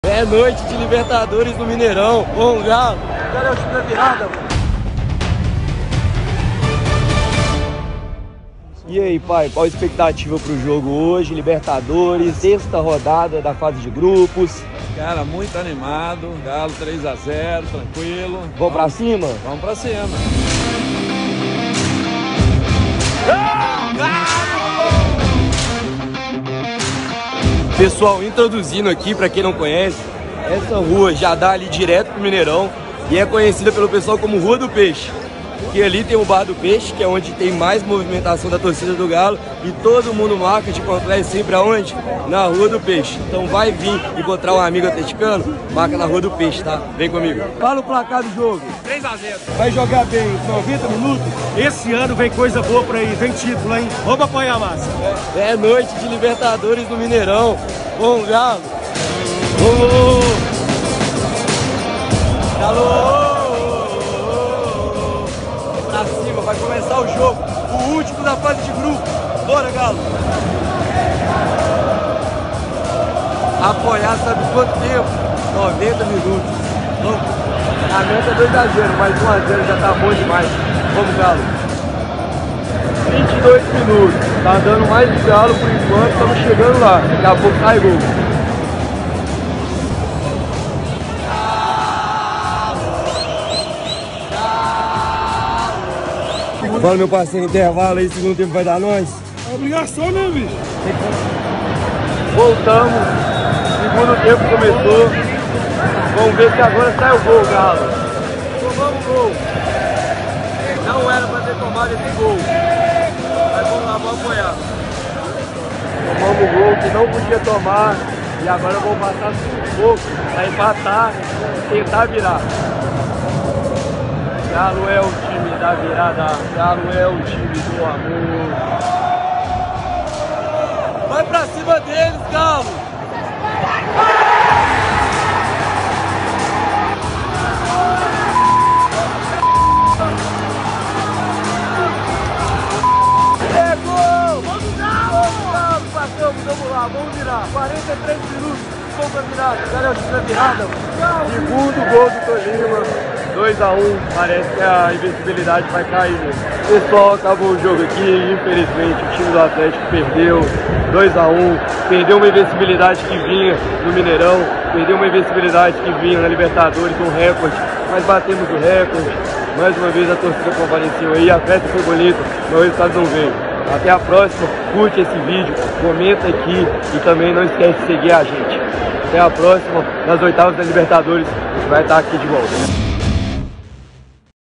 MINI VLOG GALO 1x2 TOLIMA | Noite de passar raiva no Mineirão
Fala geral do GALO o vídeo de hoje foi feito em parceira com a galera do 90min sigam eles nas redes sociais deles @90minbrasil85 pois o trabalho que vamos fazer por lá na libertadores será surreal! Registrei mais um jogaço do GALO mas dessa vez que terminou com a derrota.

Mas deu pra registrar o pré jogo, as reações da galera, o canto da torcida, o apoio mas também as insatisfações! Saímos do mineirão sem os três pontos mas com classificação garantida pras oitavas da libertadores.

⭐️ Aproveitem e se cadastrem na Estrela Bet com bônus de 100% pra quem seleciona bônus no primeiro depósito e usa esse link.